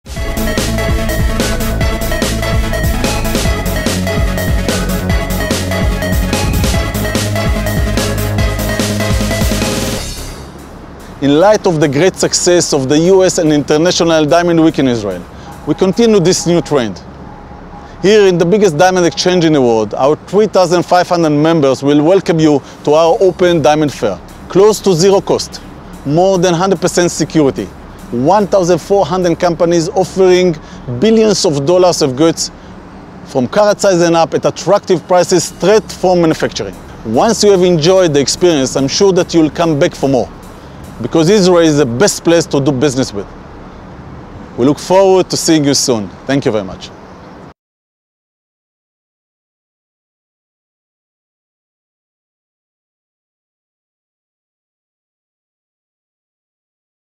יאיר סהר Invitation from IDE President Yair Sahar to the US & International Diamond Week in Israel
Invitation to the US & International Diamond Week to be held at the Israel Diamond Exchange, August 2013